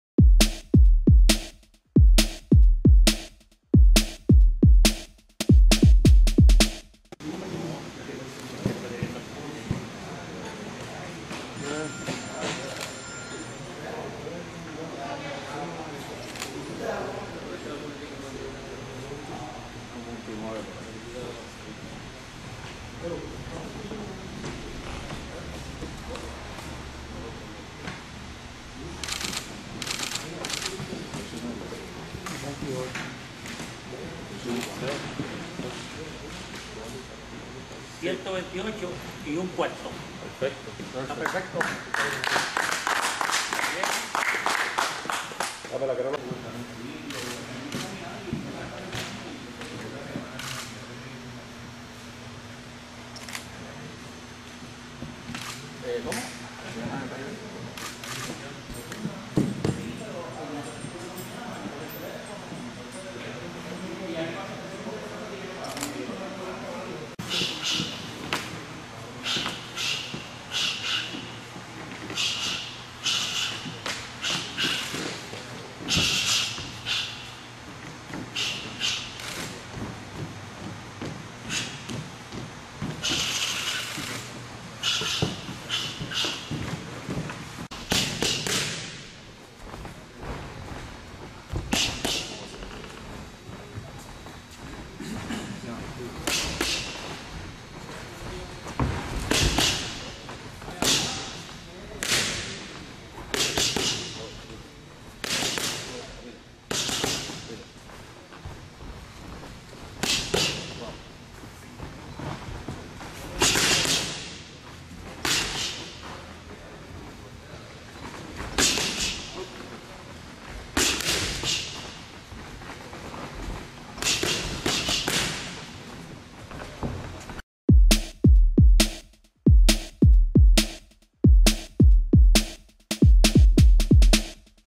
Pre-Pesaje del Retador, César "Junito" Seda
El boricua César "Junito" Seda (20-0, 15KO's), campeón NABO de la Organización Mundial de Boxeo (OMB) de las 115 libras, marco 128 y 1/4 libras durante el primer pre-pesaje para su pelea de título mundial contra a Omar Narváez (33-0-2, 19KO's) el próximo 15 de abril en Buenos Aires, Argentina.